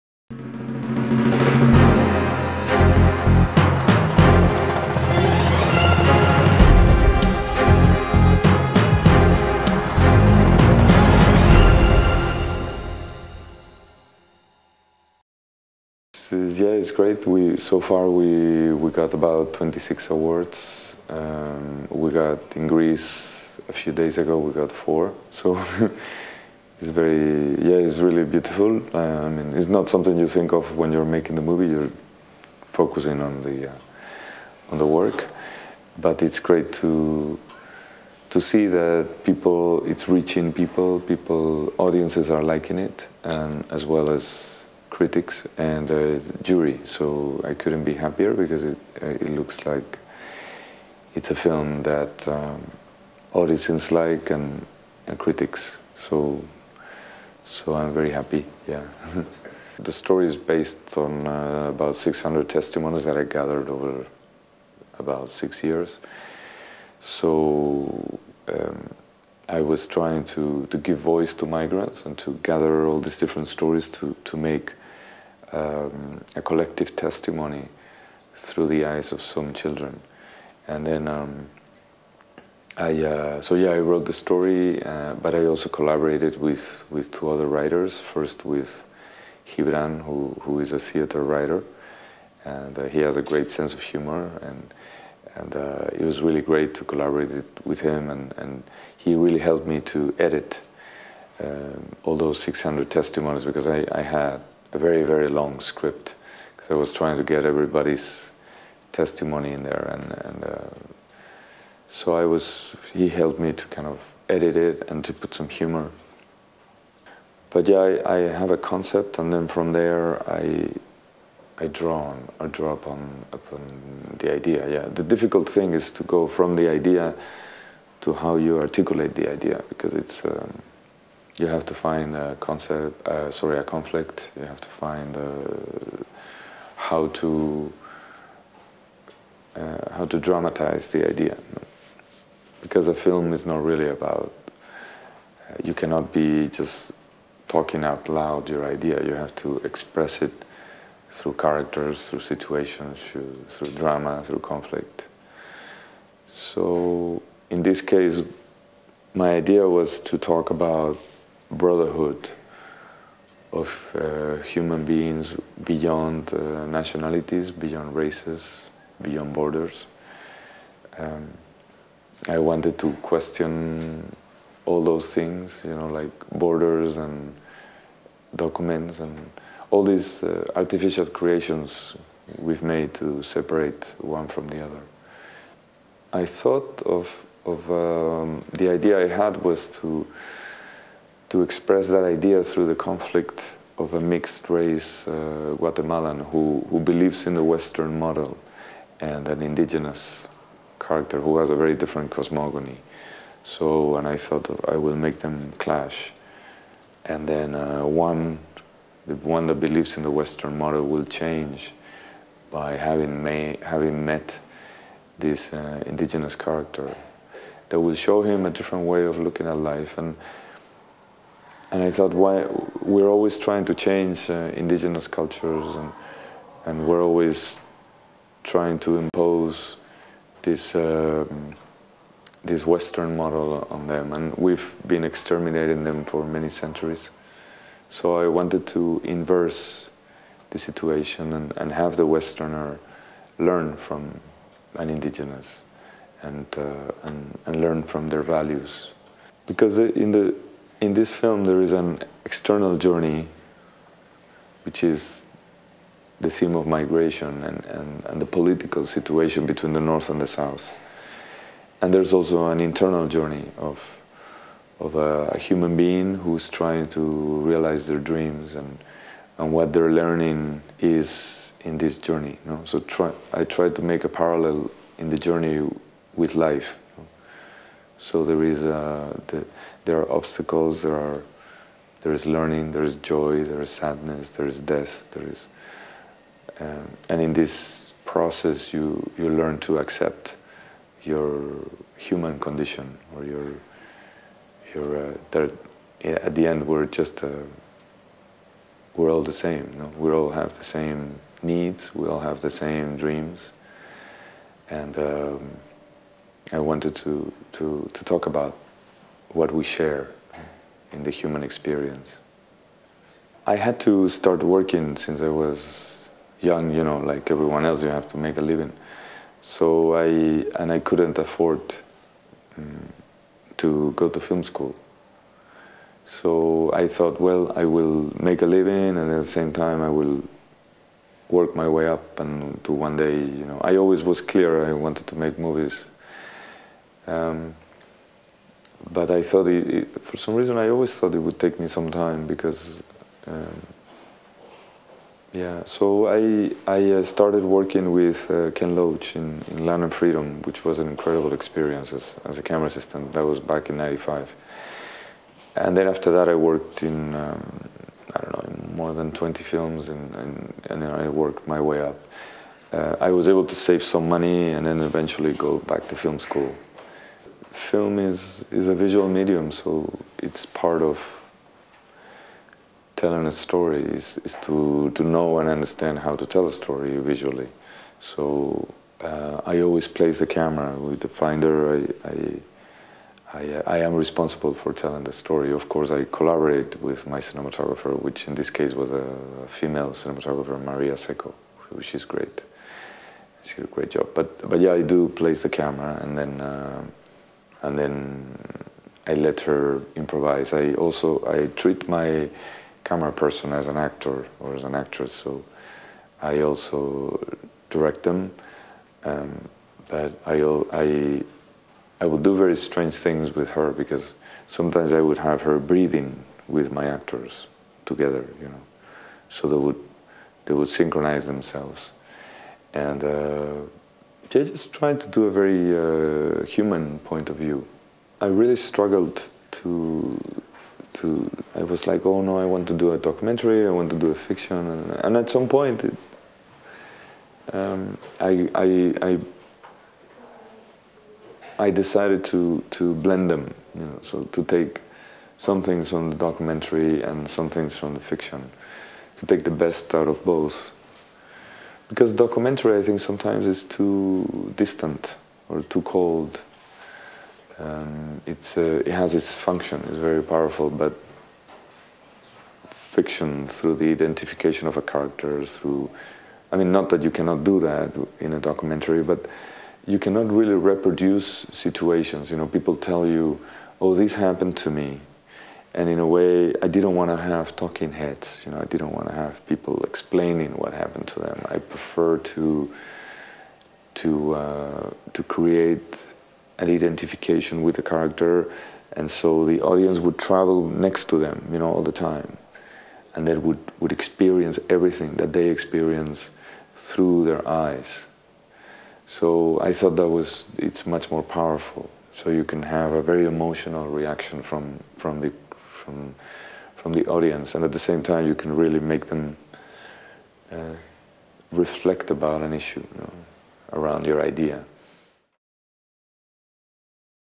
Diego Quemada Diez - Web Interview - Stockholm International Film Festival 2013
Interview with Diego Quemada Diez, director of "La Jaula De Oro".

Reporter: Leo-Jacques Jonas
Camera and editing: Jorge Pérez